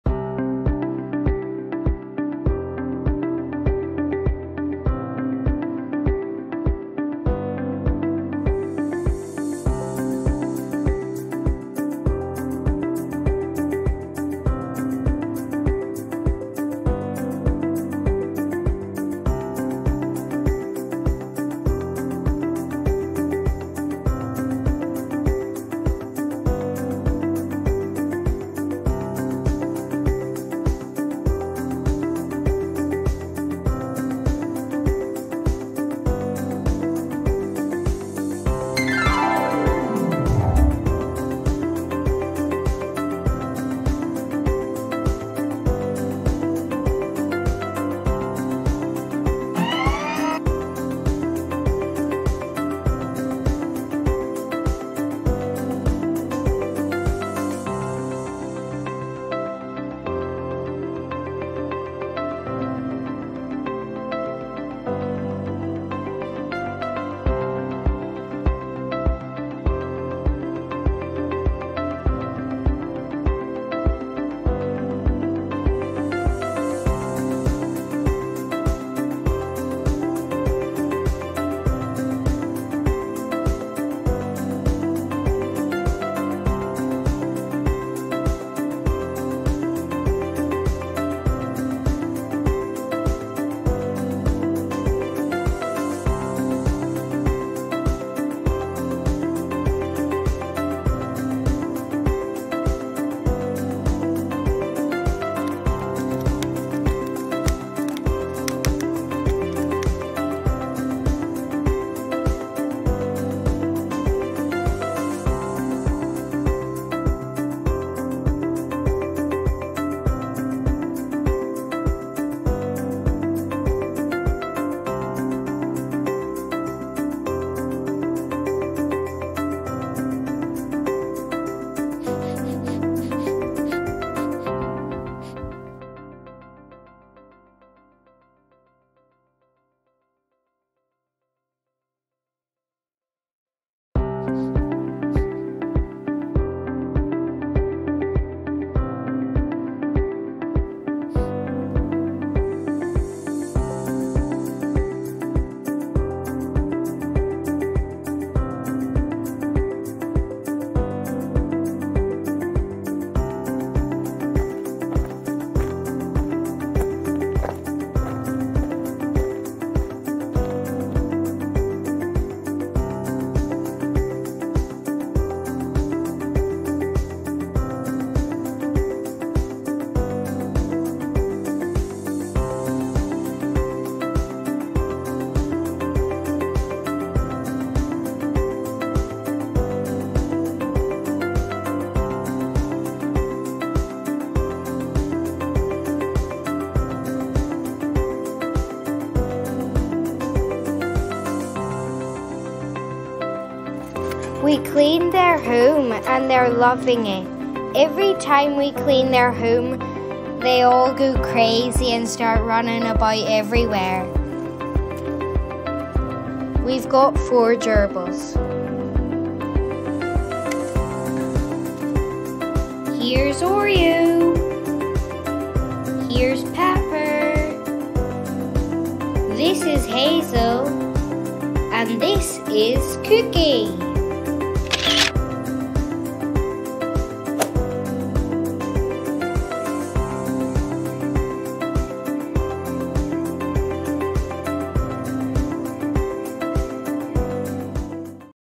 Our Gerbils are so Playful!
These wee scoots are the BEST! They are really playful, cute and so soft!

...Well, I guess you'll just have to watch the video to see what it is in a few days!